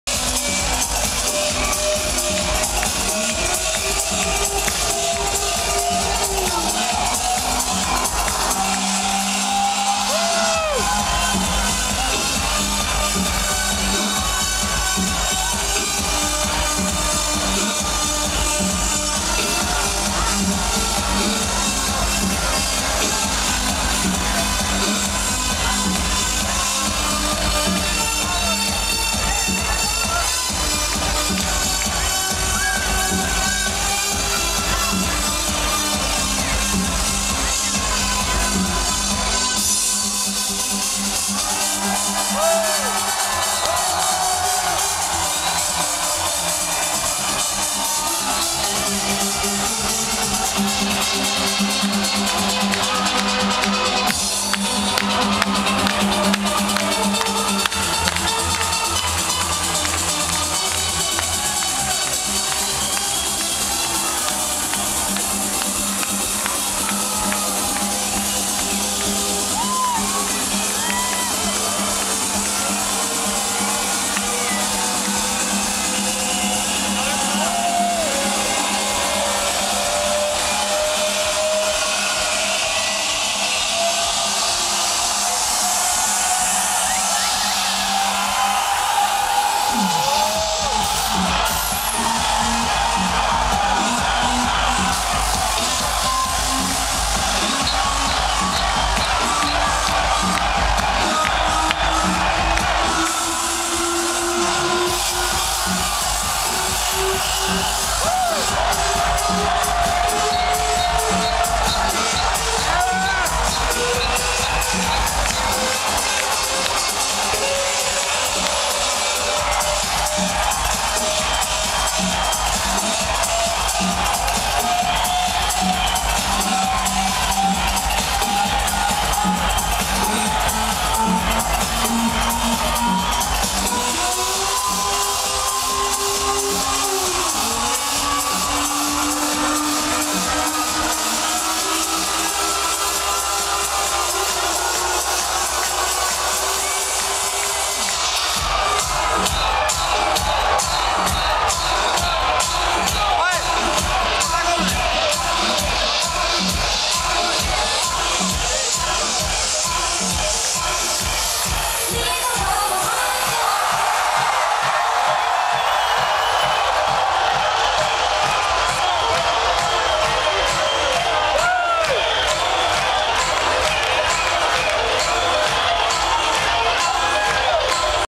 Laidback Luke @ Ultra Music Festival 2010
Laidback Luke playing at UMF 2010 in the Ibiza Arena.